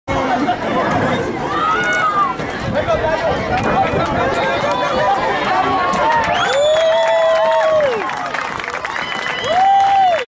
Daniel D Flores #10 TD
TD Run Rio Hondo Jr High vs Raymondville Jr High